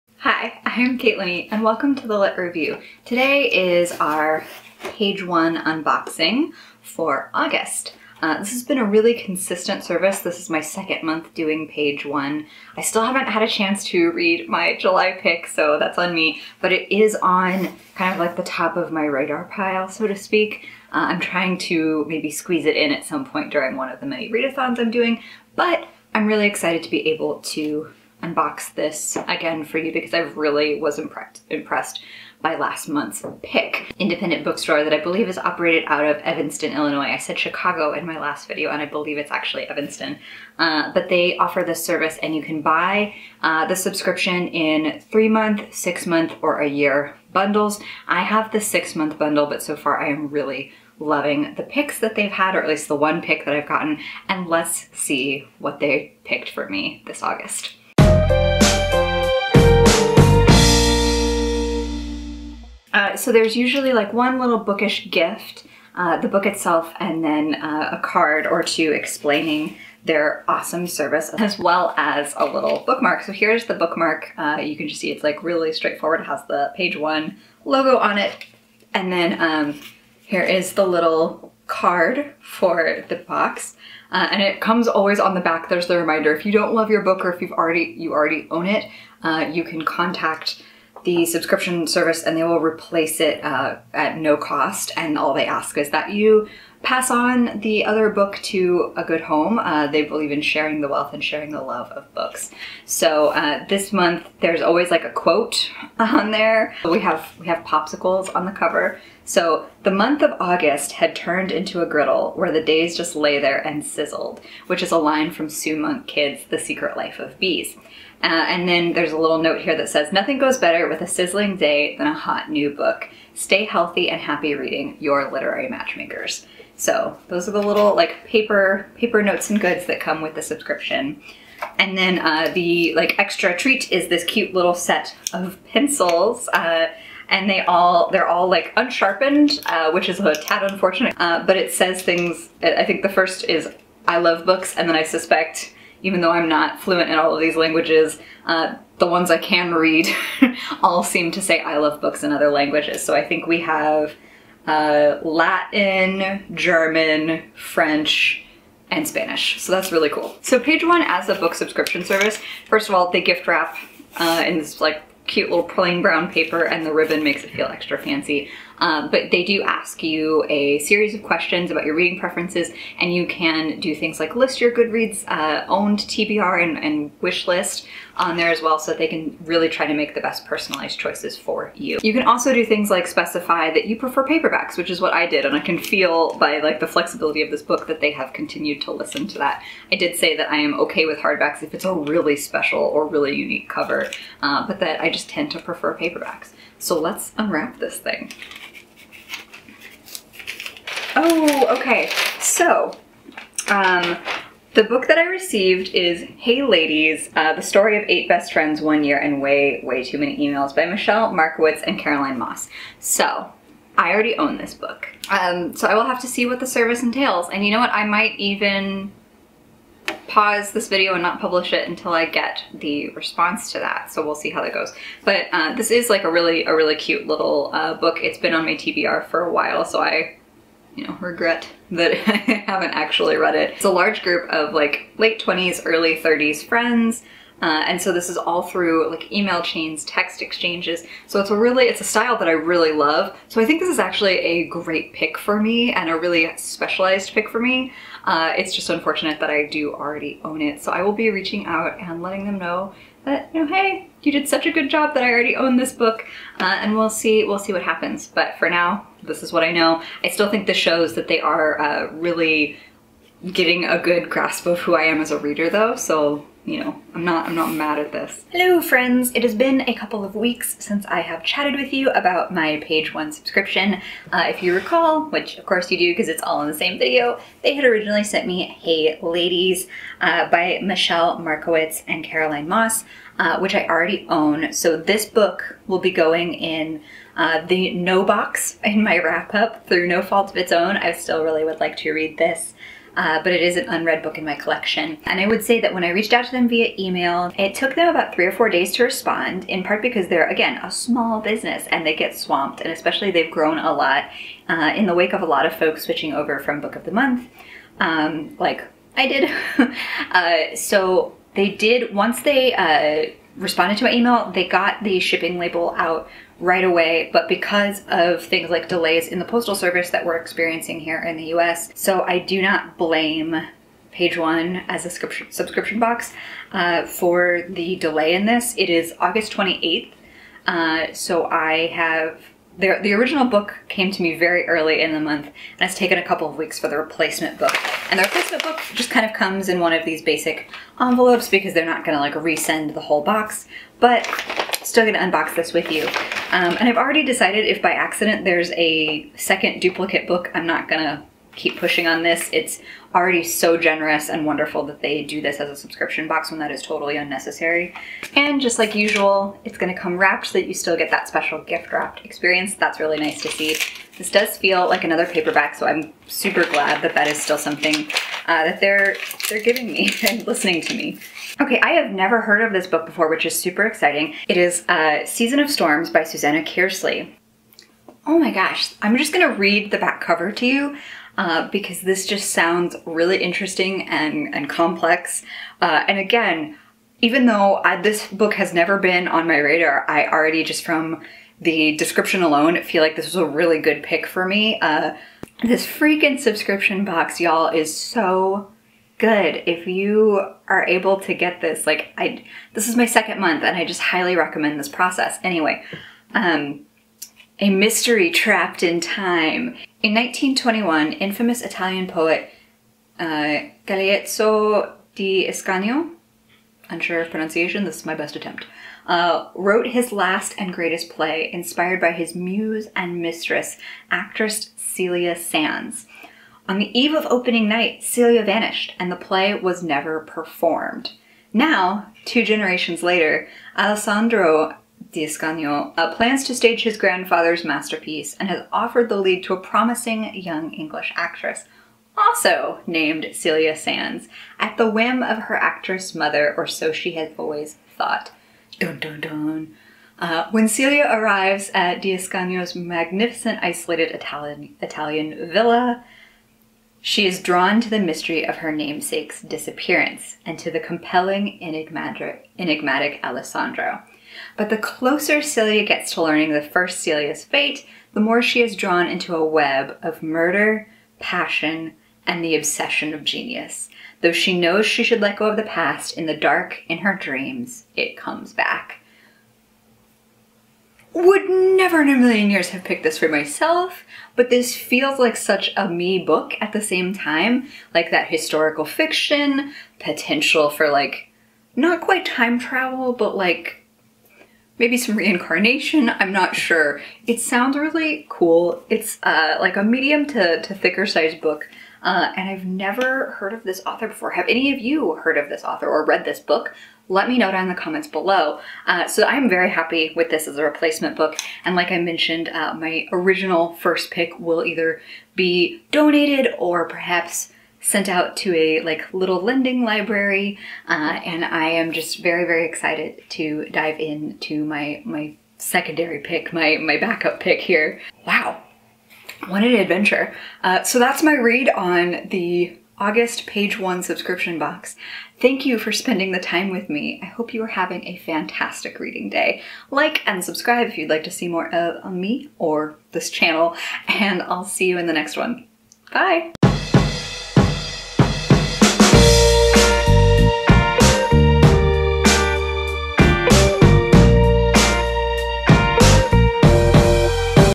Page 1 Unboxing August 2020
This is my second month as a Page 1 subscriber, and I’m still really impressed with this service. This month, my first book was a duplicate of something in my collection, but I was not disappointed by their replacement policy. As with all my subscriptions, I pay for this myself but love sharing them with you! If you’re curious about Page 1, I’ve left the details of the box down below.

The Page 1 subscription box comes in three different tiers/bundles.

3 months = $65.00 + shipping
6 months = $120.00 + shipping
1 year = 225.00 =shipping

This article links to a bunch of charities connected to Black Lives Matter and/or trans communities

Amazon Wishlist (Open through August):